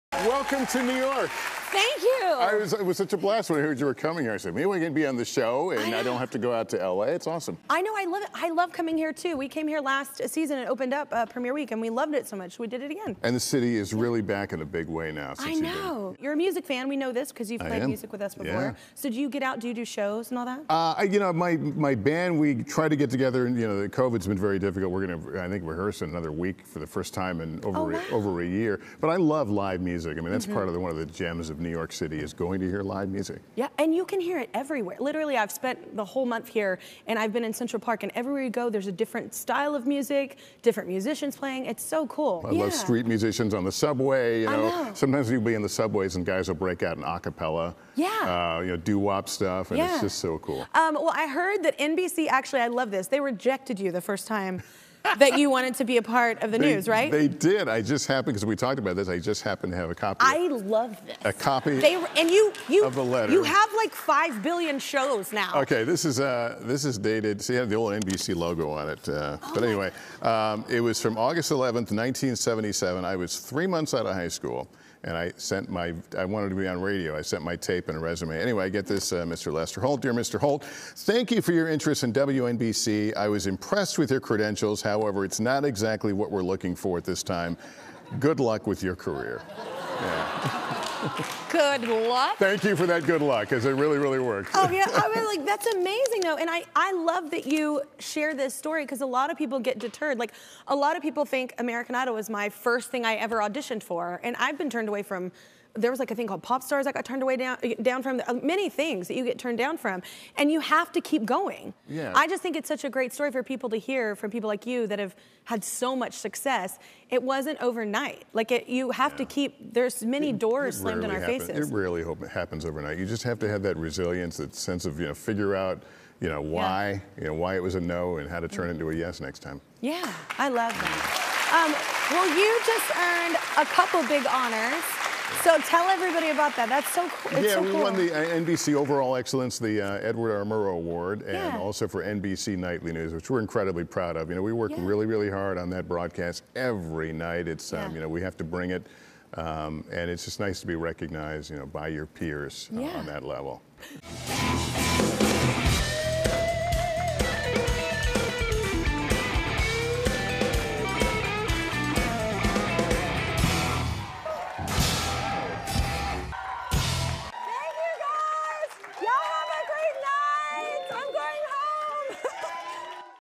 Lester Holt Kept His First Rejection Letter From NBC
"NBC Nightly News" host Lester Holt reveals that he still has a copy of the original rejection letter he received from NBC after asking for a job just three months out of high school.